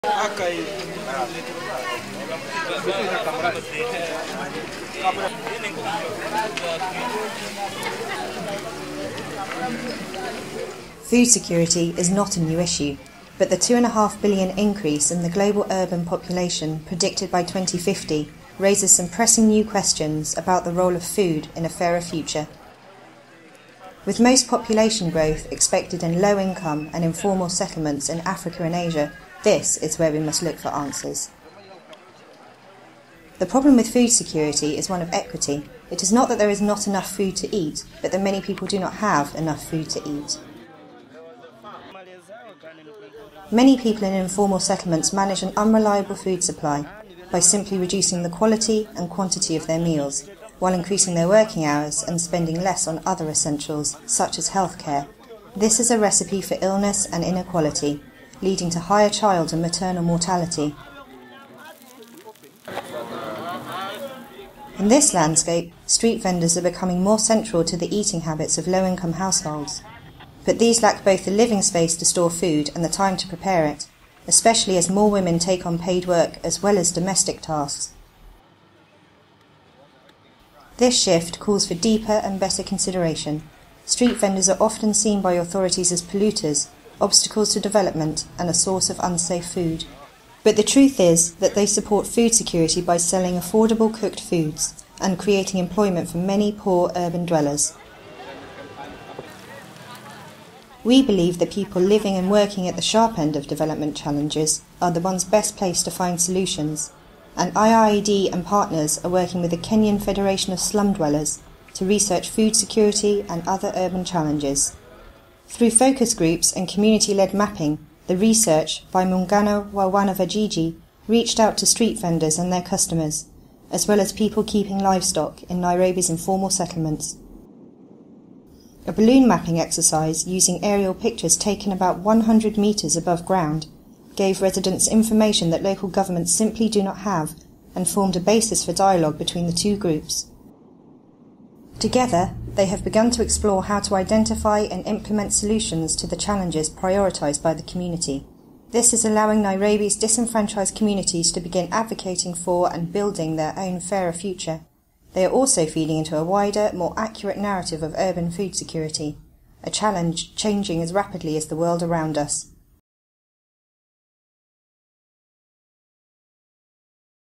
Mapping the fresh challenges of urban food security
Food security is not a new issue. But the 2.5 billion increase in the global urban population predicted by 2050 raises some pressing new questions about the role of food in a fairer future.

With most population growth expected in low-income and informal settlements in Africa and Asia, this is where we must look for answers.

This film, produced for the 2014/15 annual report of the International Institute for  Environment and Development, highlights the role played by street vendors, and how they support food security by selling affordable cooked foods and creating employment for many poor urban dwellers.

IIED and partners are working with a Kenyan federation of slum dwellers, Muungano wa Wanavijiji, to research food security and other urban challenges.

Film produced by Paolo Cravero and Matt Wright (IIED); voiceover by Katharine Mansell (IIED); photos by Sohel Ahmed.